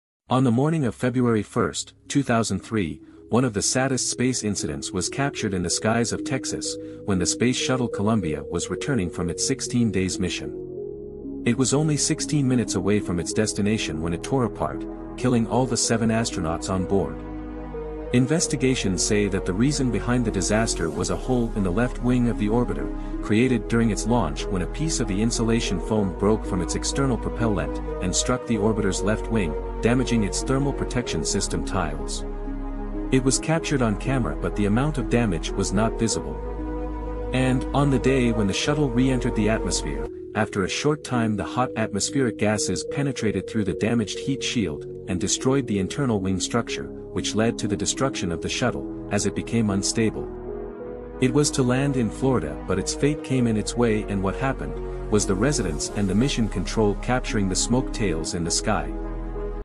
The Space Shuttle Columbia disaster!
You can
             to my channel if you want! 😄

This video is about the Space Shuttle Columbia disaster!

What happened to the space shuttle Columbia?
What caused the shuttle destruction?

On the morning of February 1st 2003, one of the saddest space incidents was captured in the skies of Texas and Louisiana, when the space shuttle Columbia was returning from its 16 days mission. It was only 16 minutes away from its destination(Florida) when it tore apart, killing all the seven astronauts on board!
Investigations say that the reason behind the disaster was a hole in the left wing of the orbiter, created during its launch when a piece of the insulation foam broke from its external propellent, and struck the orbiter's left wing, damaging its thermal protection system tiles! It was captured on camera but the amount of damage was not visible, and also NASA didn't address it as a big concern!
But on the day, when the shuttle re-entered the atmosphere, after a short time the hot atmospheric gases penetrated through the damaged heat shield, and destroyed the internal wing structure, which led to the destruction of the Shuttle Columbia, as it became unstable!
It was to land in Florida but it's fate came in its way and what was happened, was the residents and the mission control capturing the smoke tales in the sky,
it never made it!